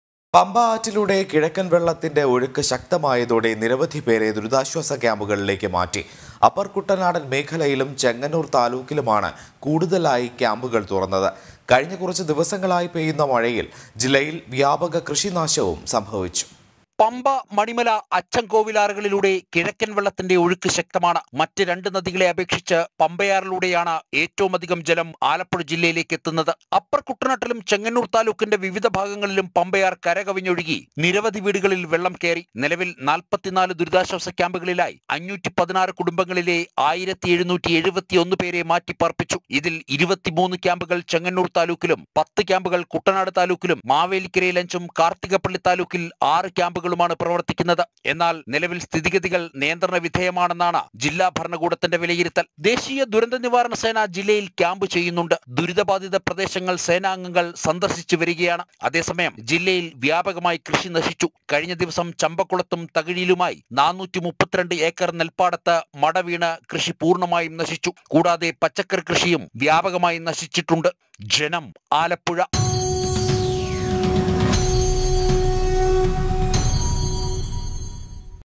നിരവധിപേരെ ദുരിതാശ്വാസക്യാമ്പുകളിലേക്ക് മാറ്റി
പമ്പ ആറ്റിലൂടെ കിഴക്കൻ വെള്ളത്തിന്‍റെ ഒഴുക്ക് ശക്തമായതോടെ നിരവധിപേരെ ദുരിതാശ്വാസക്യാമ്പുകളിലേക്ക് മാറ്റി